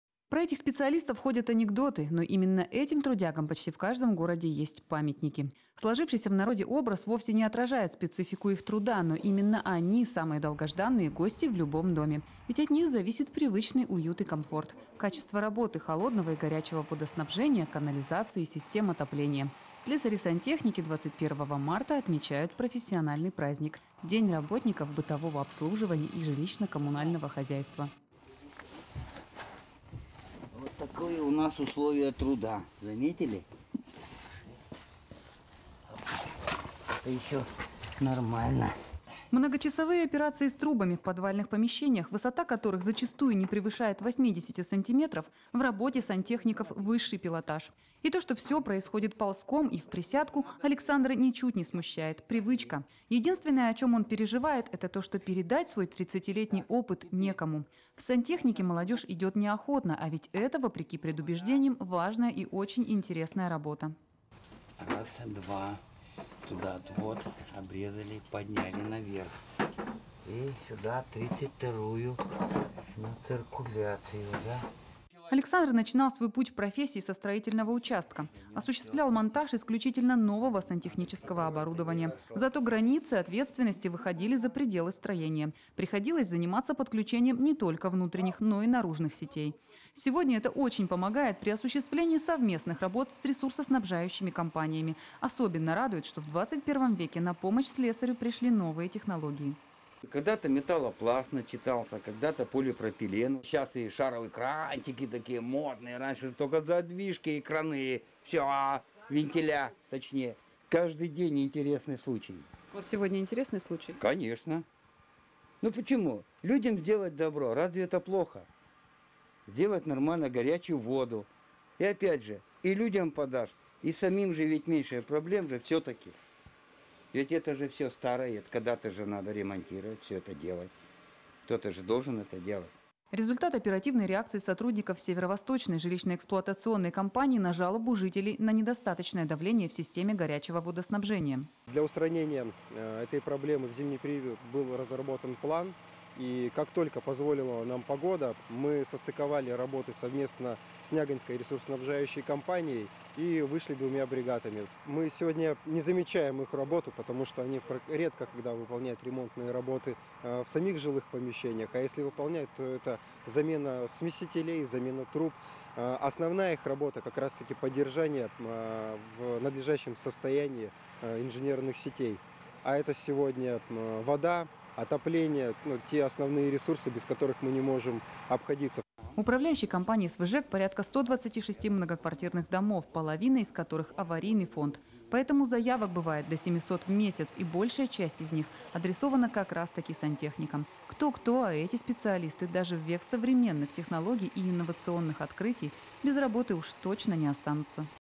В Нягани рассказали, как важен труд сантехников
Про этих специалистов ходят анекдоты, но именно этим трудягам почти в каждом городе есть памятники. Сложившийся в народе образ вовсе не отражает специфику их труда, но именно они - самые долгожданные гости в любом доме, ведь от них зависит привычный уют и комфорт: качество работы холодного и горячего водоснабжения, канализации, систем отопления.